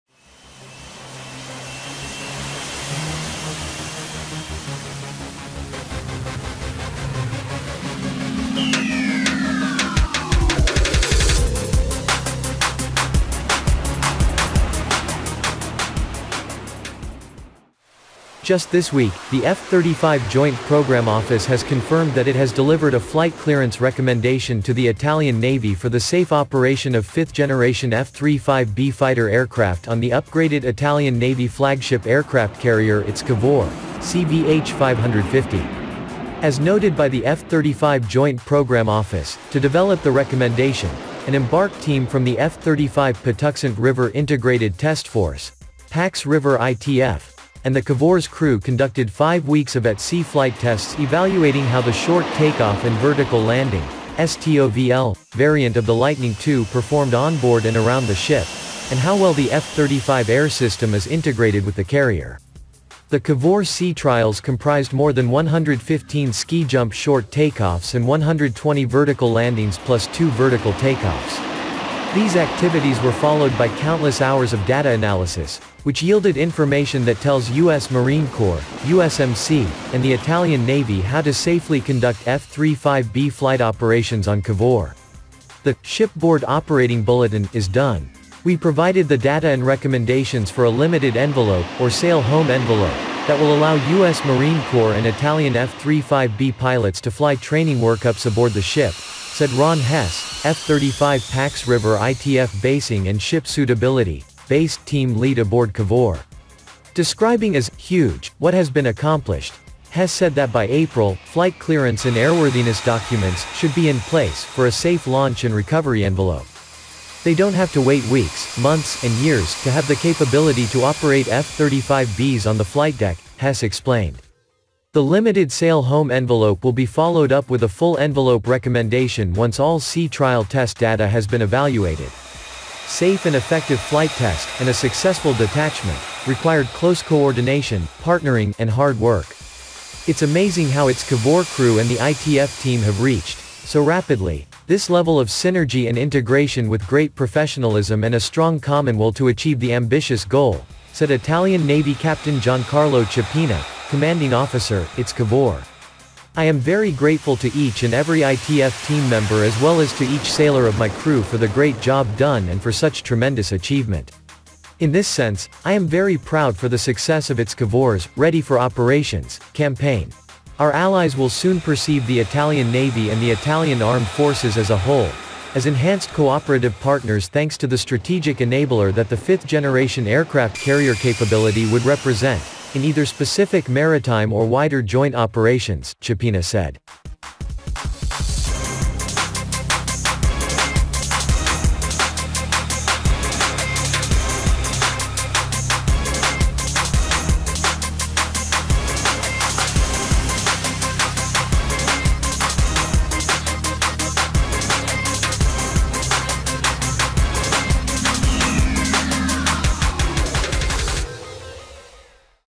Operation of the F-35B fighters on the Italian carrier is ready
Just this week, the F-35 Joint Program Office has confirmed that it has delivered a flight clearance recommendation to the Italian Navy for the safe operation of fifth-generation F-35B fighter aircraft on the upgraded Italian Navy flagship aircraft carrier ITS Cavour (CVH 550).

As noted by the F-35 Joint Program Office, to develop the recommendation, an embarked team from the F-35 Patuxent River Integrated Test Force (Pax River ITF) and the Cavour’s crew conducted five weeks of at-sea flight tests evaluating how the short takeoff and vertical landing (STOVL) variant of the Lightning II performed onboard and around the ship, and how well the F-35 air system is integrated with the carrier.

The Cavour Sea Trials comprised more than 115 ski jump short takeoffs and 120 vertical landings plus two vertical takeoffs. These activities were followed by countless hours of data analysis, which yielded information that tells U.S. Marine Corps (USMC) and the Italian Navy how to safely conduct F-35B flight operations on Cavour.

“The (shipboard operating bulletin) is done. We provided the data and recommendations for a limited envelope, or sail home envelope, that will allow U.S. Marine Corps and Italian F-35B pilots to fly training workups aboard the ship,” said Ron Hess, F-35 Pax River ITF Basing and Ship Suitability (BASS) team lead aboard Cavour.

Describing as “huge” what has been accomplished, Hess said that by April, flight clearance and airworthiness documents “should be in place” for a safe launch and recovery envelope. “They don’t have to wait weeks, months, and years” to have the capability to operate F-35Bs on the flight deck, Hess explained. The limited sail home envelope will be followed up with a full envelope recommendation once all Sea Trial test data has been evaluated.

Safe and effective flight test, and a successful detachment, required close coordination, partnering, and hard work.

“It’s amazing how ITS Cavour crew and the ITF team have reached, so rapidly, this level of synergy and integration with great professionalism and a strong common will to achieve the ambitious goal,” said Italian Navy Capt. Giancarlo Ciappina, commanding officer, ITS Cavour. “I am very grateful to each and every ITF team member as well as to each sailor of my crew for the great job done and for such tremendous achievement.

“In this sense, I am very proud for the success of ITS Cavour’s “Ready for Operations” campaign: our allies will soon perceive the Italian Navy and the Italian Armed Forces as a whole, as enhanced cooperative partners thanks to the strategic enabler that the fifth generation aircraft carrier capability would represent, in either specific maritime or wider joint operations,” Ciappina said.